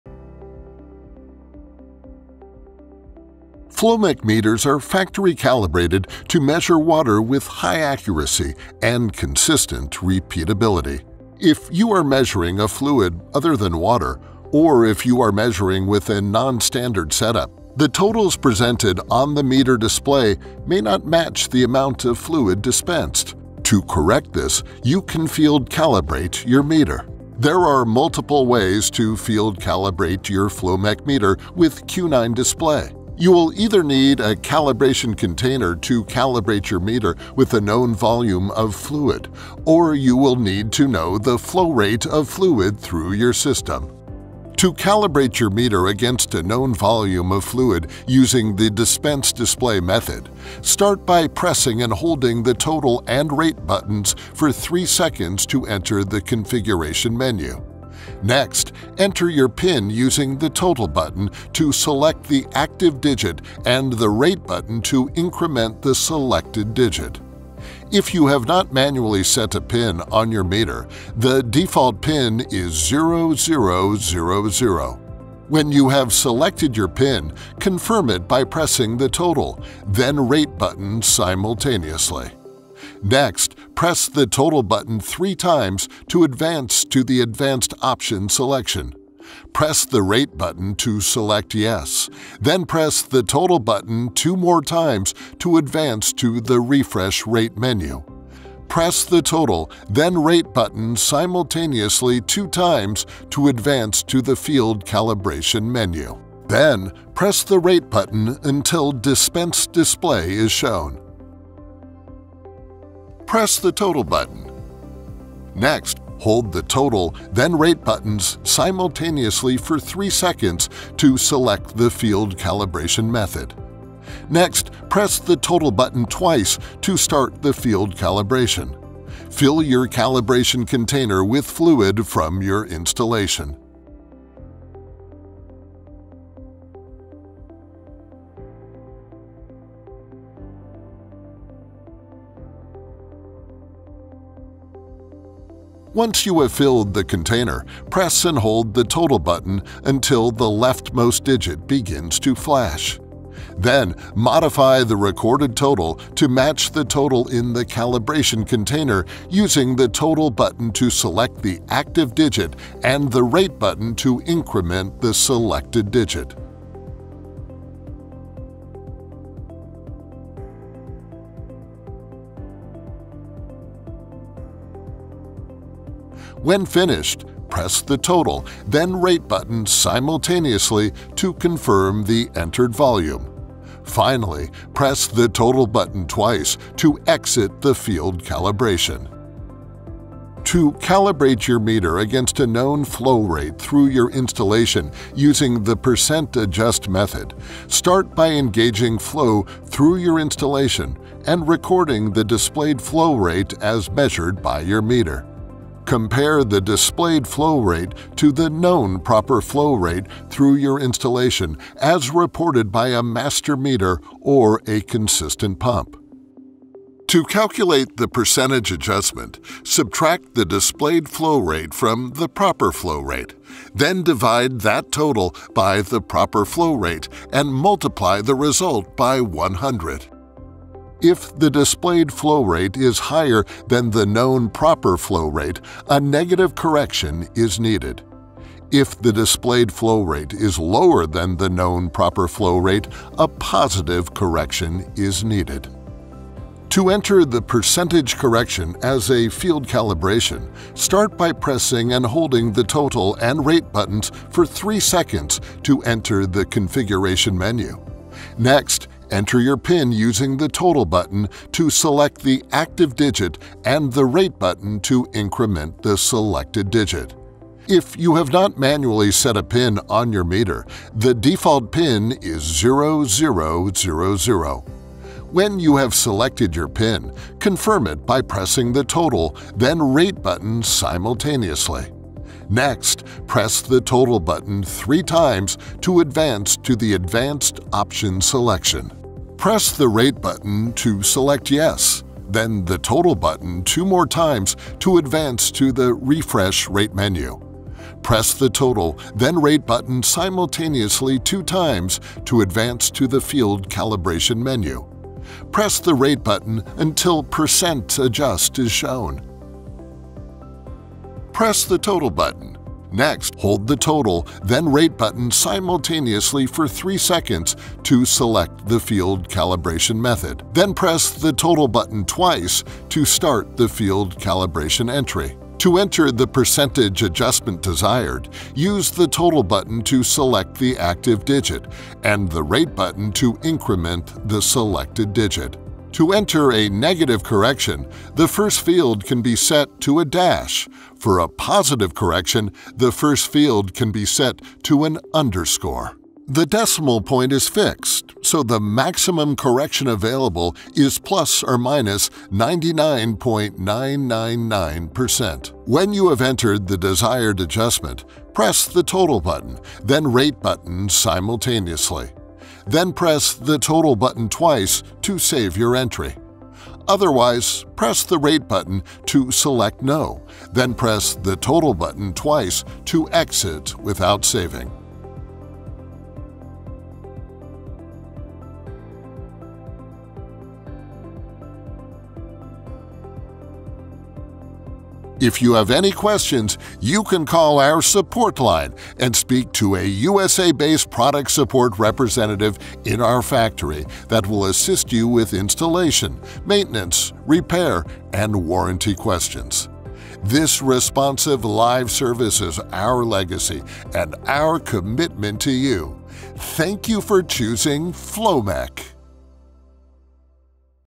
FLOMEC Q9 How To Field Calibrate for your Fluid
A quick and easy how-to guide on how to field calibrate your FLOMEC Q9 computer display. If you are measuring a fluid other than water or if you are measuring with a non standard set up the totals presented on the meter display may not match the fluid dispensed. To correct this, you can field calibrate your meter.

For detailed instructions please see our Owner's Manual:
For Agency Approved Meters:

If you need assistance with your replacement or with installation, contact our GPI Customer support department. We will be happy to assist you.

Great Plains Industries Customer Support Department